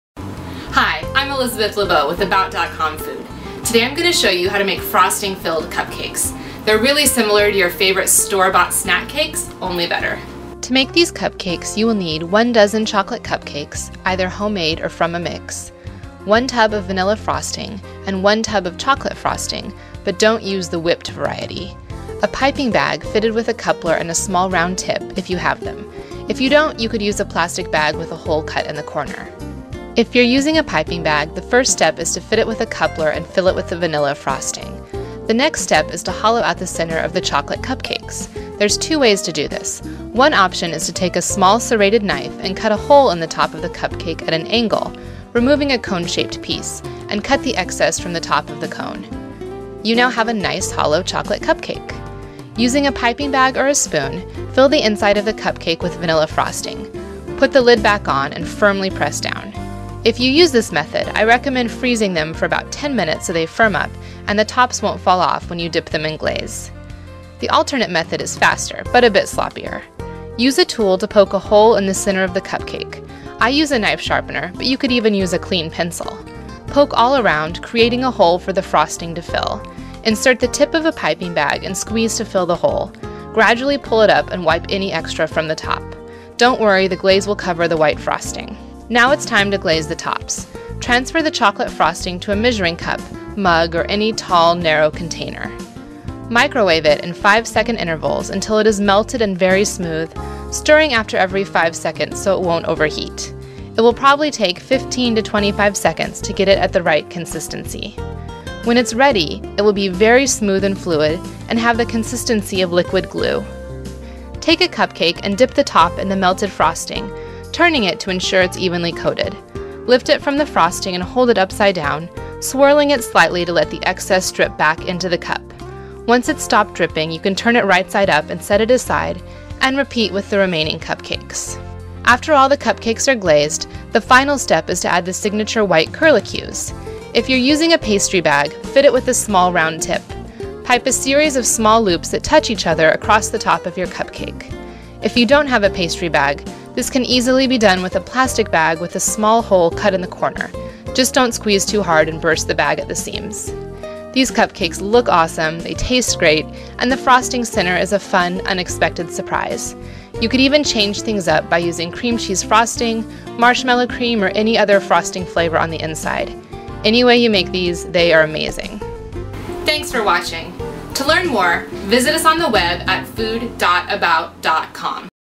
Make Frosting-Filled Cupcakes
I recovered another video tutorial from the deep dark depths of my old Windows 8.1 laptop. This was also downloaded in 2013 and has not seen the light of day since it was removed from YouTube in 2014.